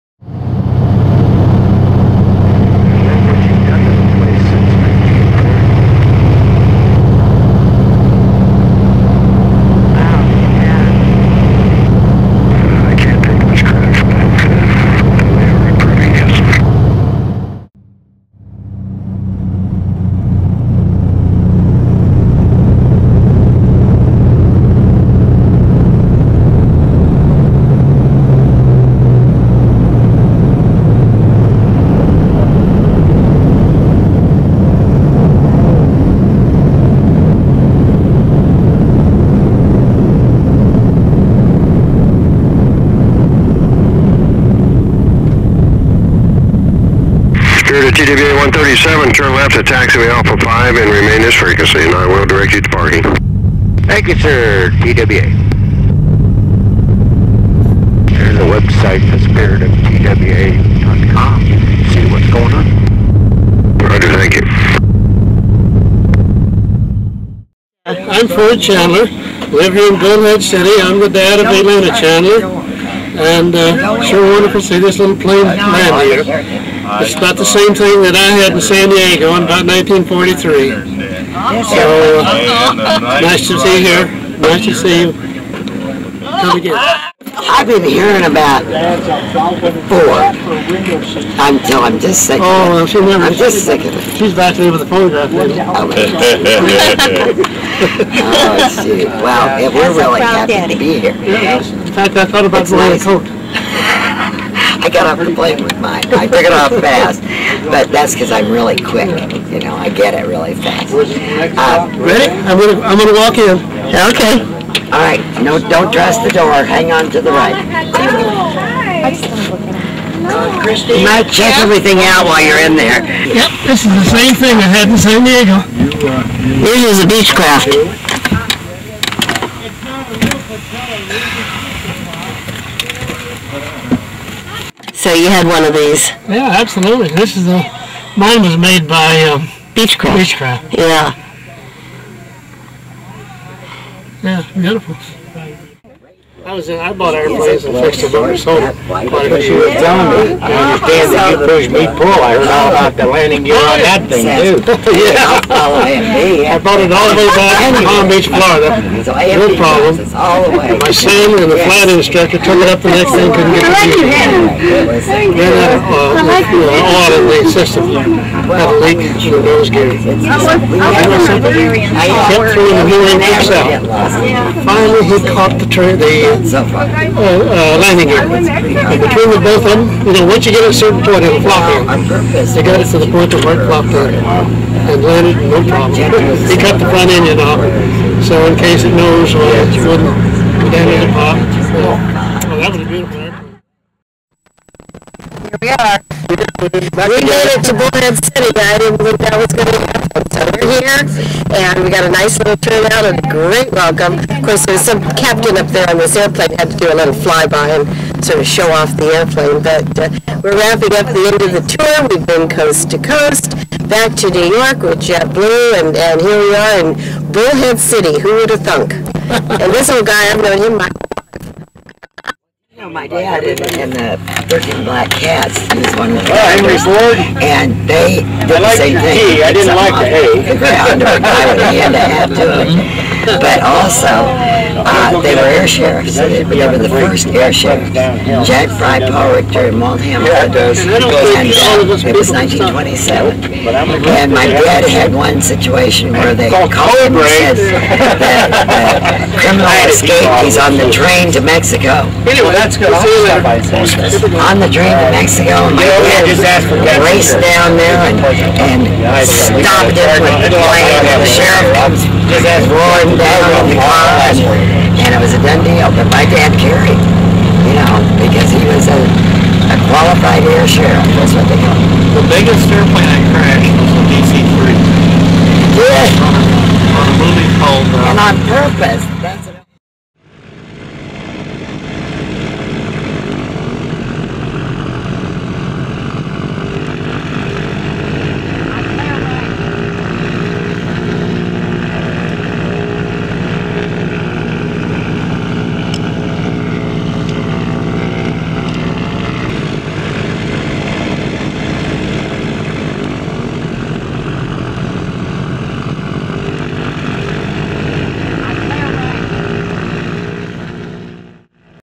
Coast to Coast Spirit of TWA Bullhead City, AZ
Westbound stop in Bullhead City, refueling and a quick visit to say hi to my father Ford Chandler.